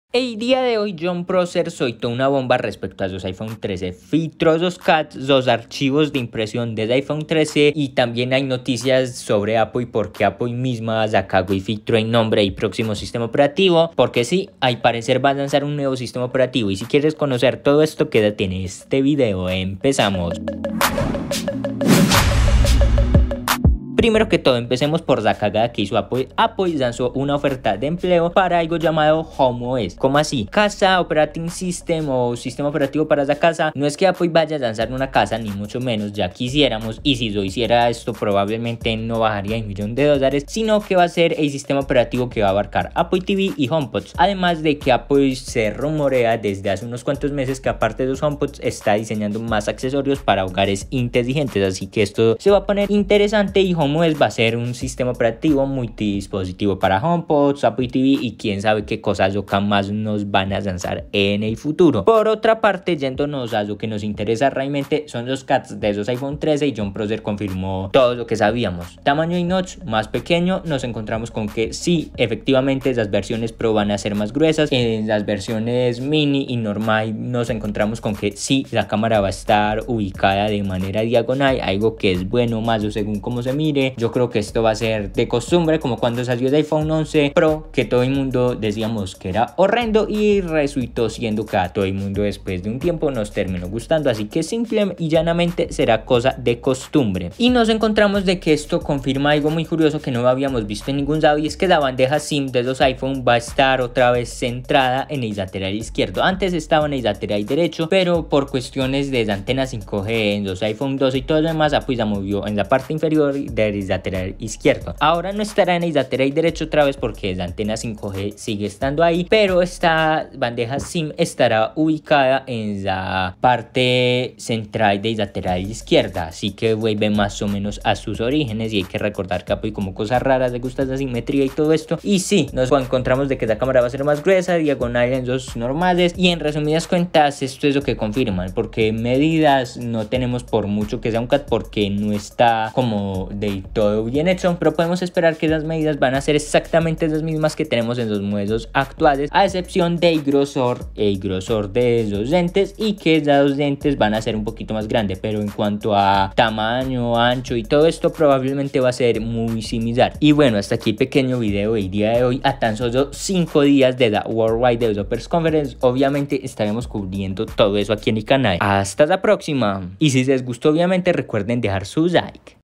FILTRAN EL iPhone 13 Detalladamente Y Nuevo SISTEMA OPERATIVO / Ultimas Noticias
Jon Prosser filtro los planos CAD del próximo iPhone y sin duda alguna esto nos confirma una gran cantidad de rumores y filtraciones que habían sobre este dispositivo